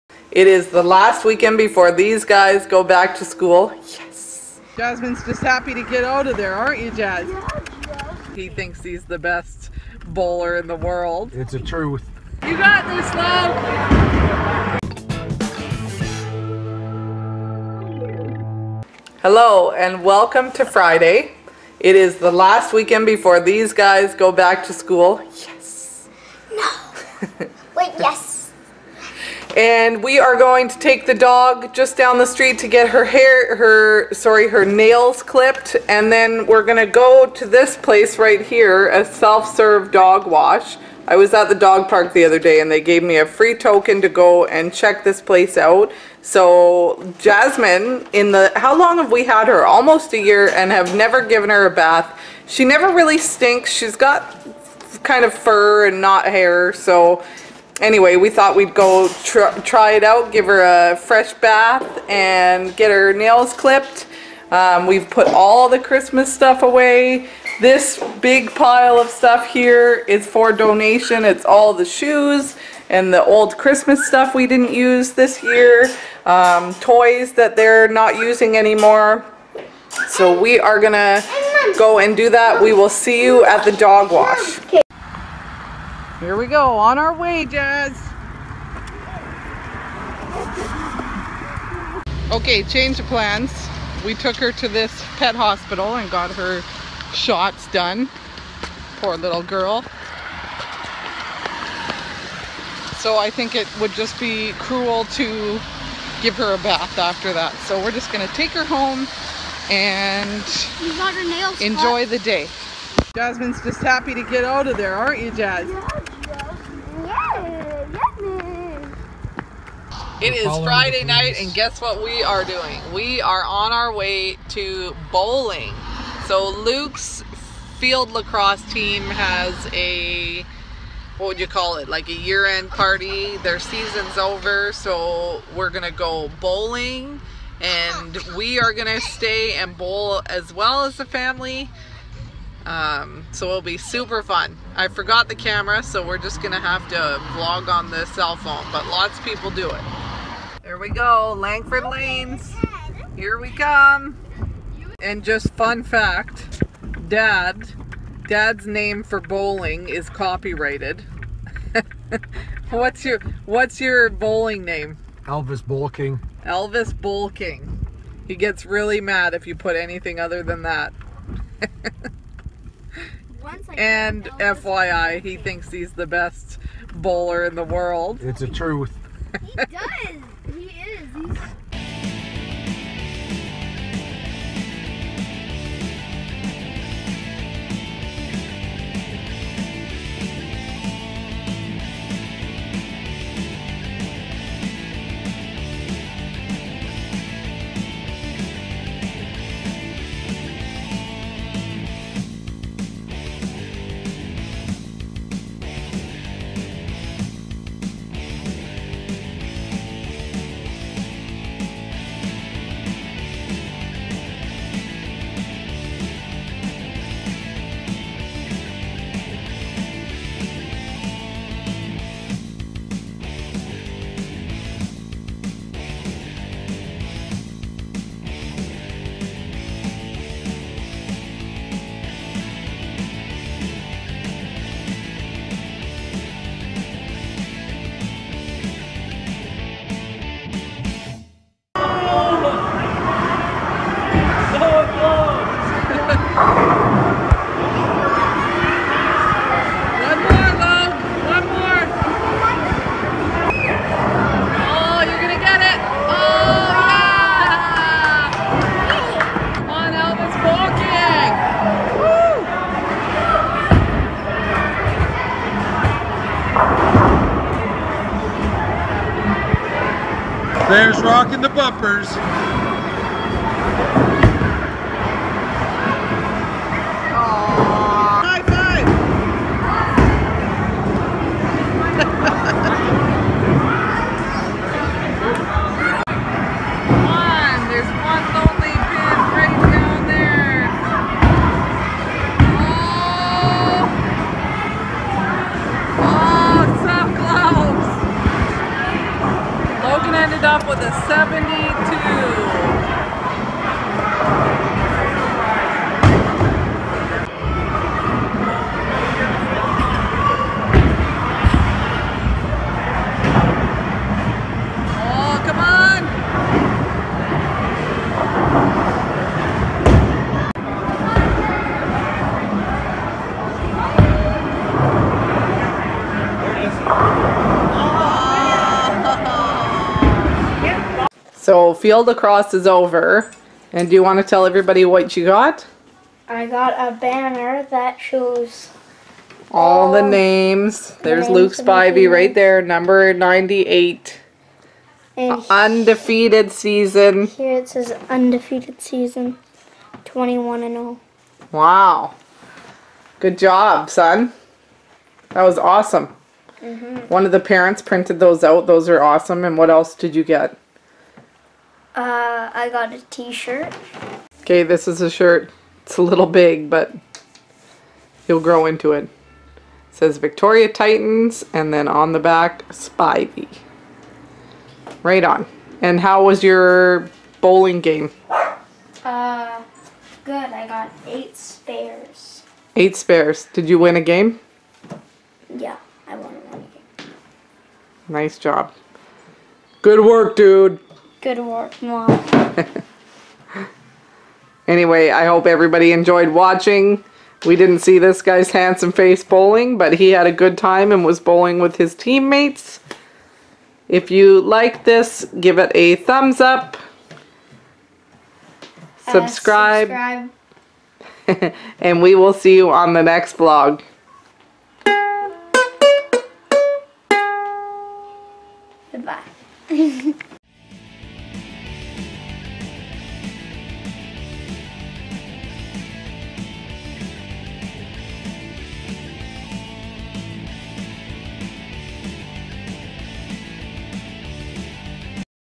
Bowling Family Fun With Elvis Bowl King - Vlog #151
We had a great time at the bowling alley with the kids. Some good family fun was had and Logan did amazing.Enjoy!